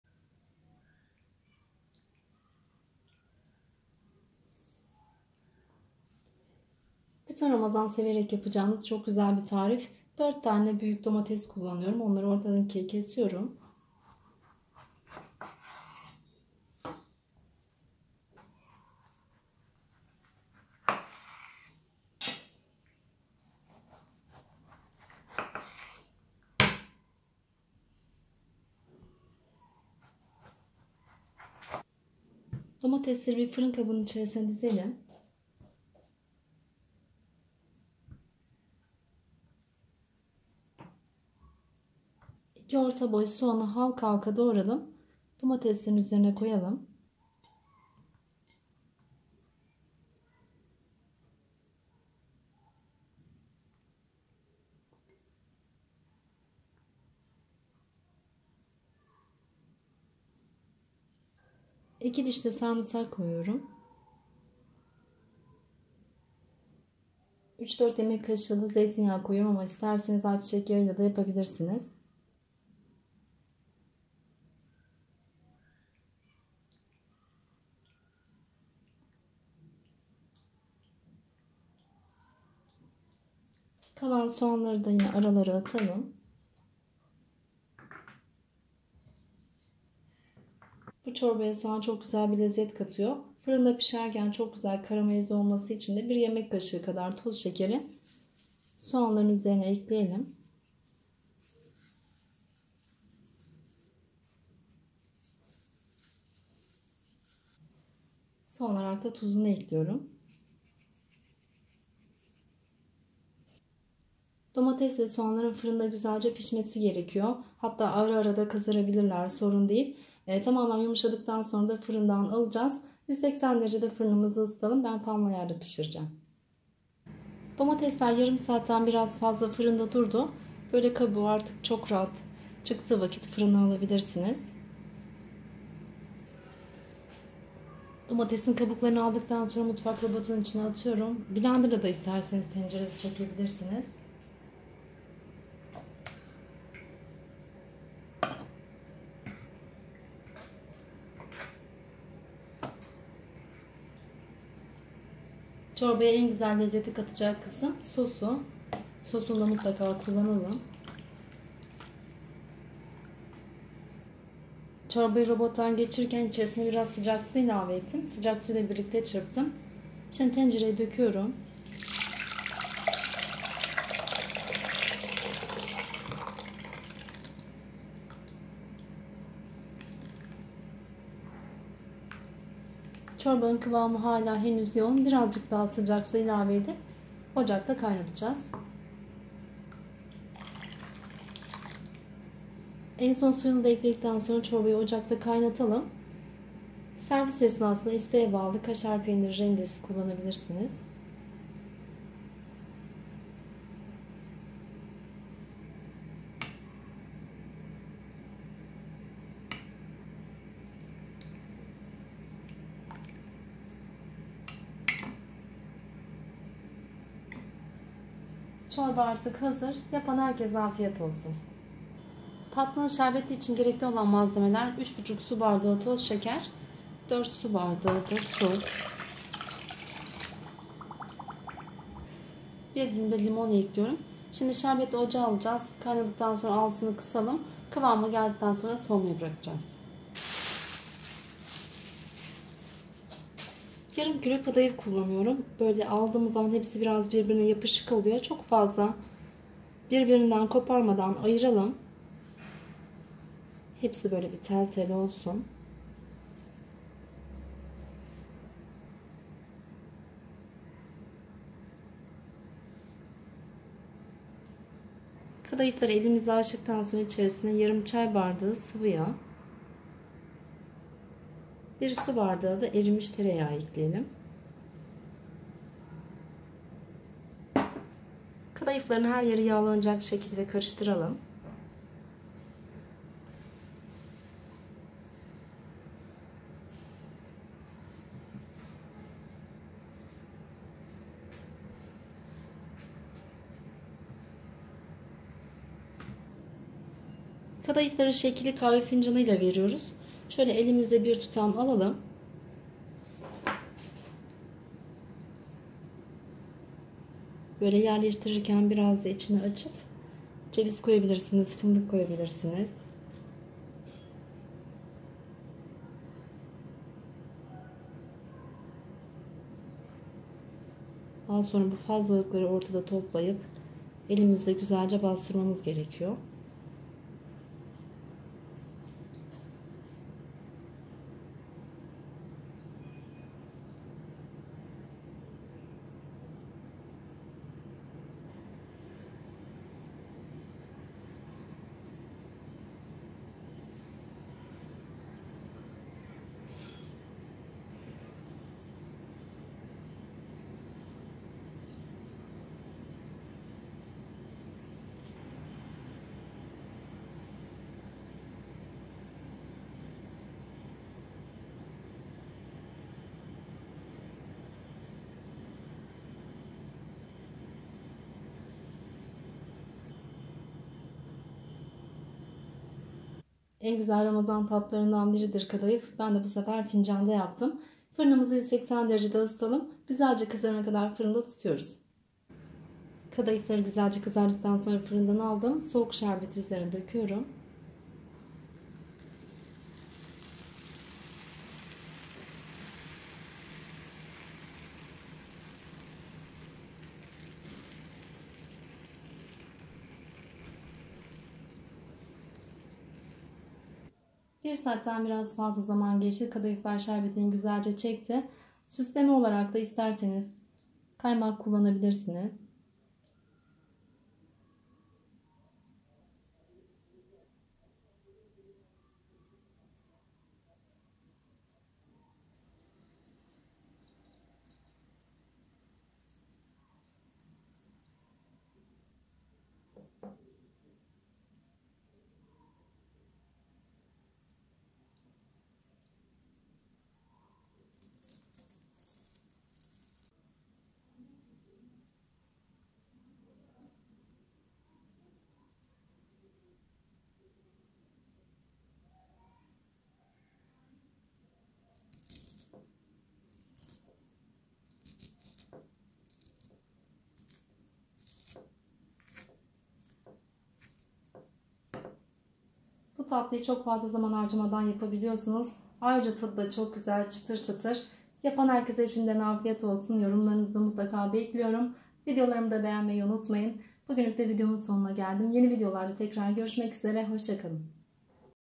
SADECE 10 DAKİKADA HERŞEY HAZIR..REKOR DENENEN 2 TARİF..KÖZ DOMATES ÇORBASI, FİNCAN KADAYIFI"
HER GÜN YEPYENİ TARİFLER LEZZETİ_ASK SAYFASINDA..